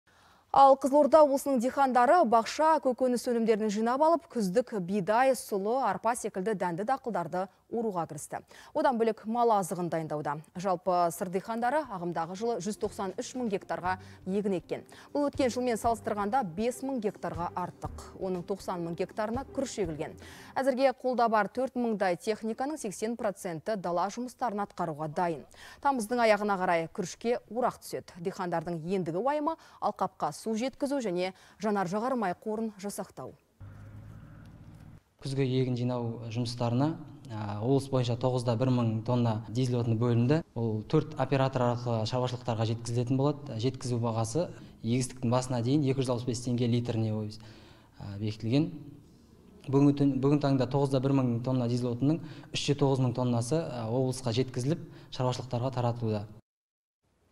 Қызылорда облысында егінге орақ түсті
Қызылорда облысының диқандары бақша, көкөніс өнімдерін жинап алып, күздік бидай, сұлы, арпа секілді дәнді дақылдарды оруға кірісті. Одан бөлек мал азығын дайындауда. Жалпы Сыр диқандары ағымдағы жылы 193 мың га егiн еккен.